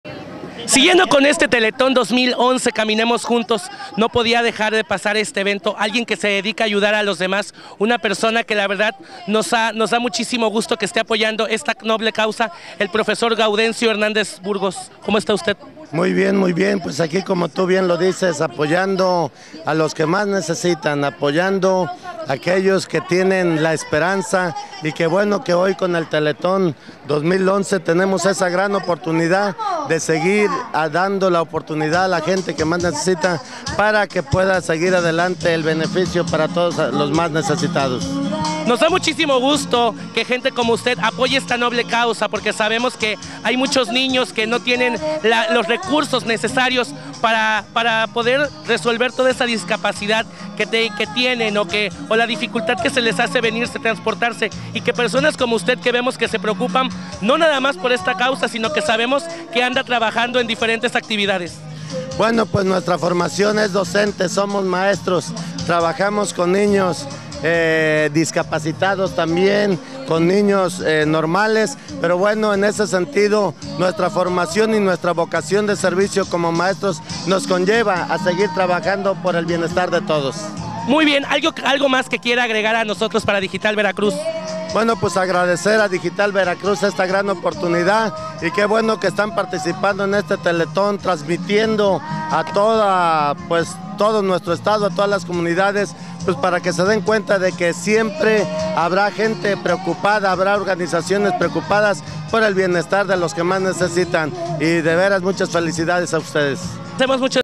profesor Gaudencio
Entrevista con el Profesor Gaudencio Hernández Burgos, Presidente de Nacidos con Luz en Poza Rica Ver., En el Evento del Teletón 2011